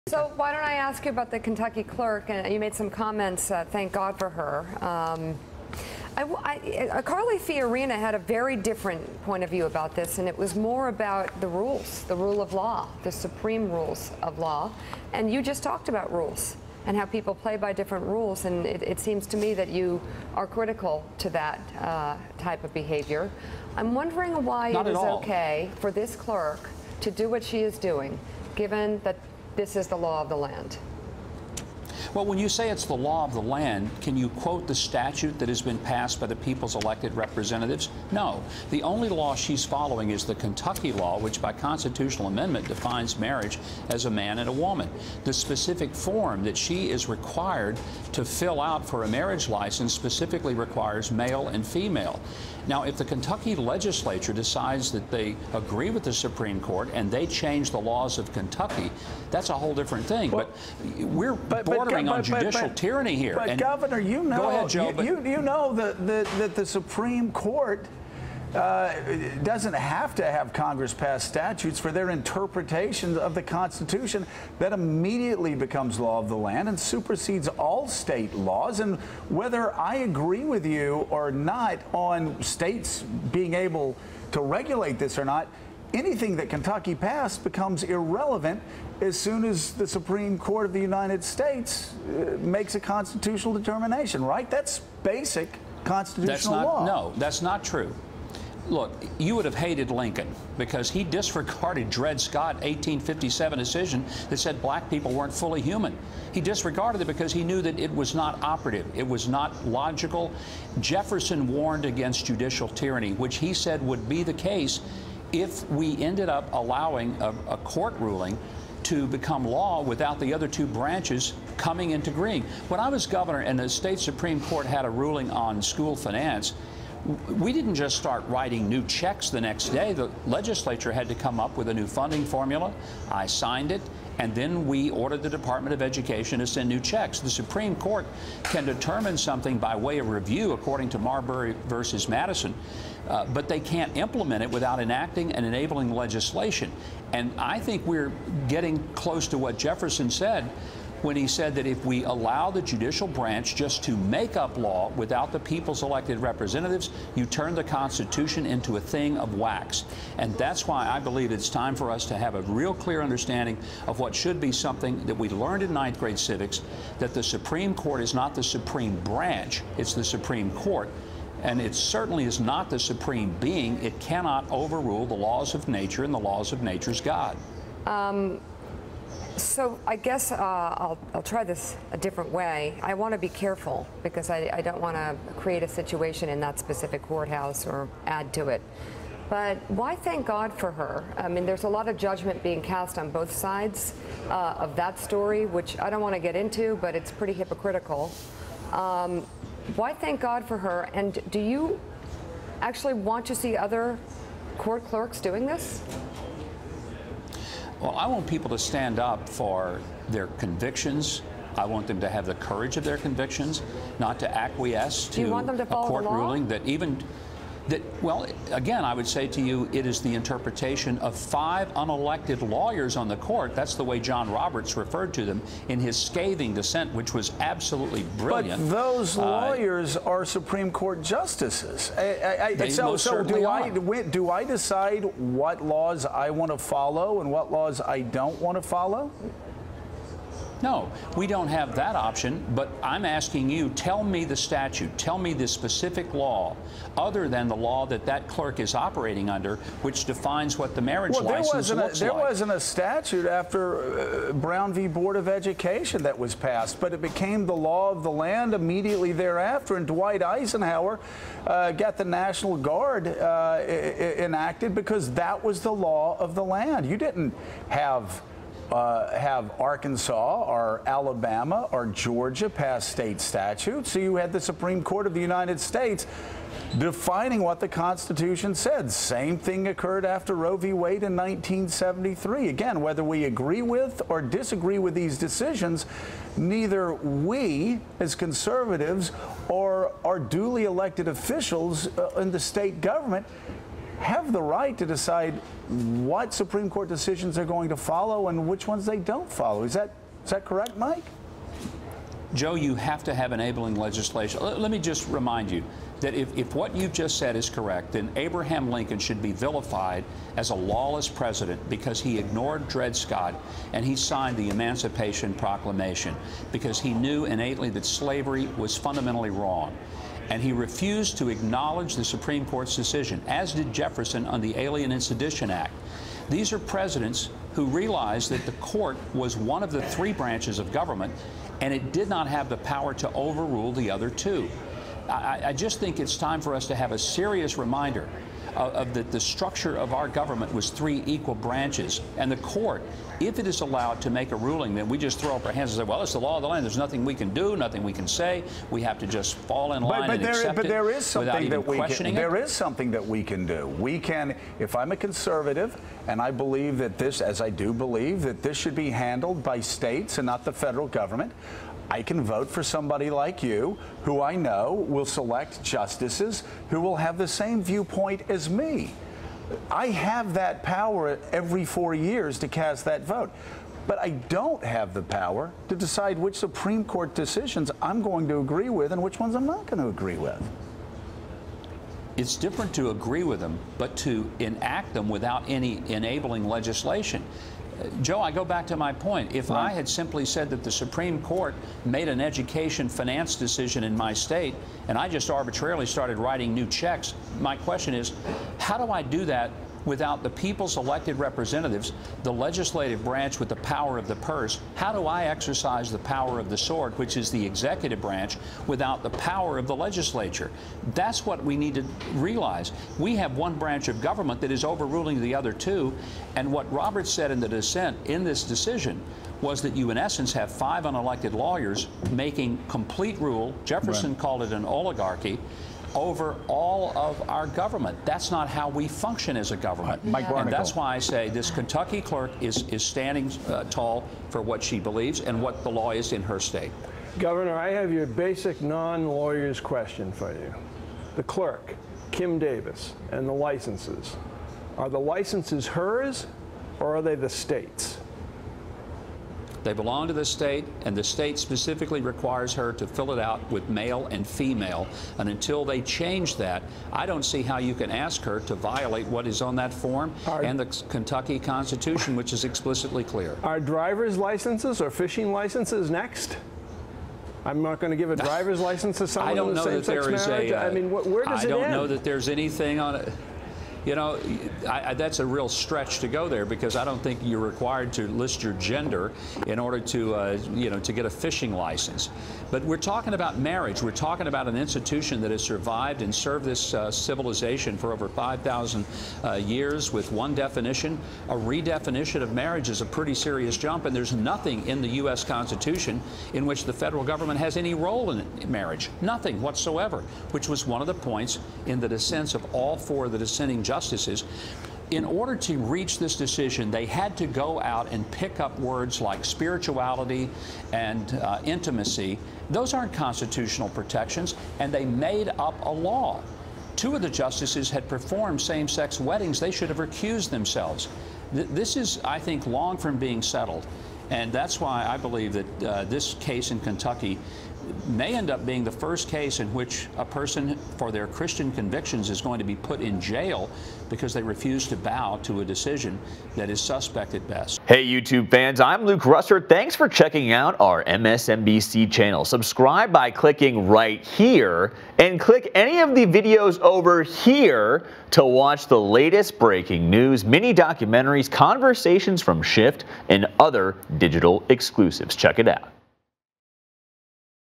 Mike Huckabee: Marriage Is Between A Man And Woman In Kentucky | MSNBC
2016 GOP presidential candidate Mike Huckabee discusses Kentucky clerk Kim Davis and why the Supreme Court's ruling on marriage equality has no authority in the state without enabling legislation.